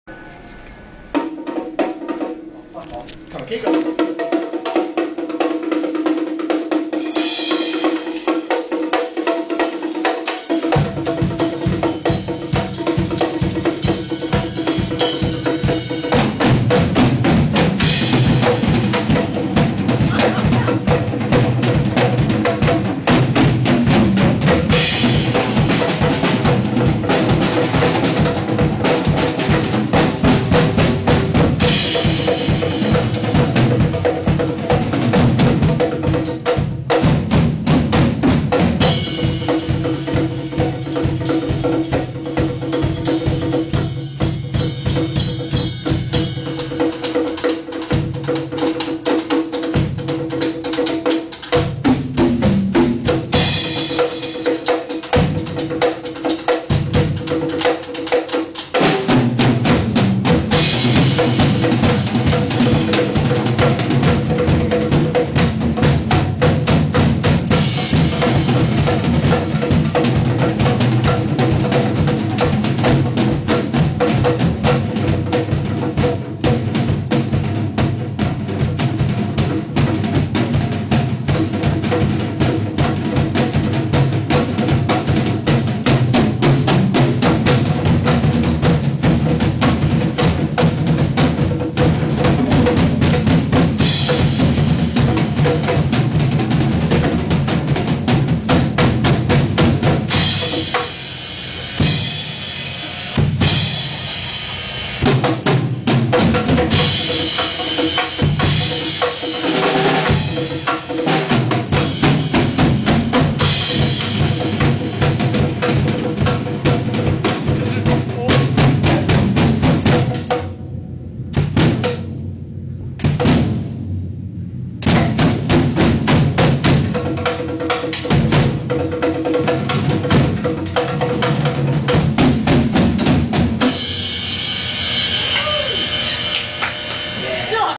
Awsome drum song thing
tyler and micheal playing drums at school